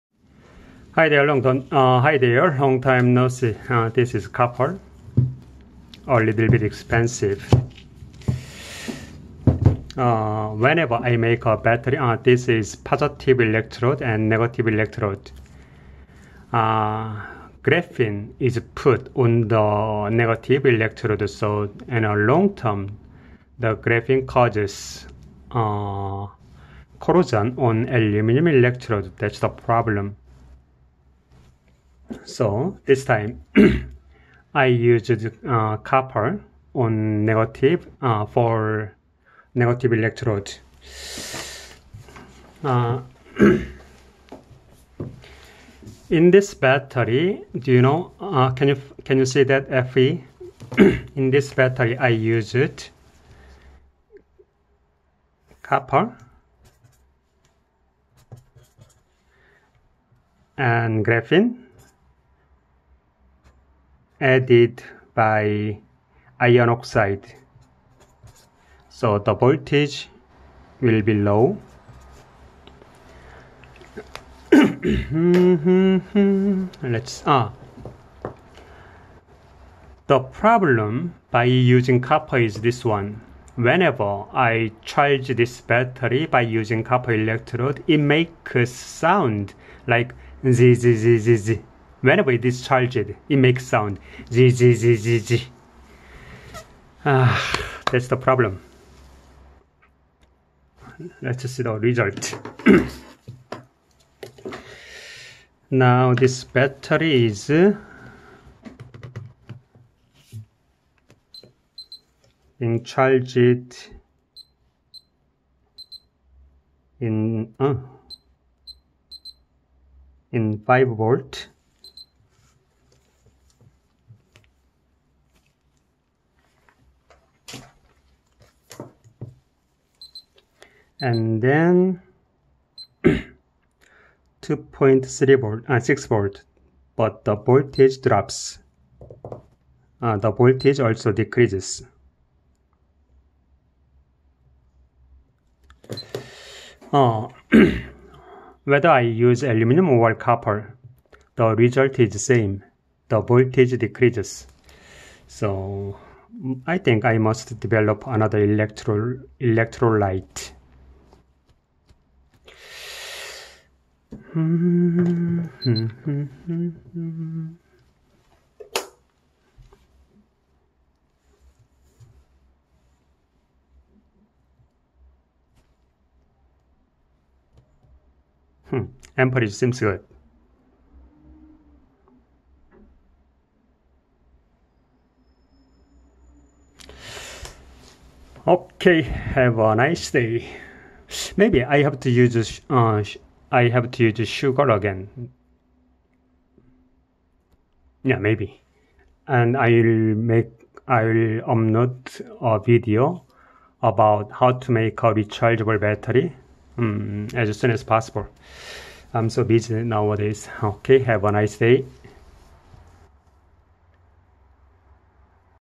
Solid state battery: using copper for a negative electrode
For this battery, I used graphene + Feo on negative electrode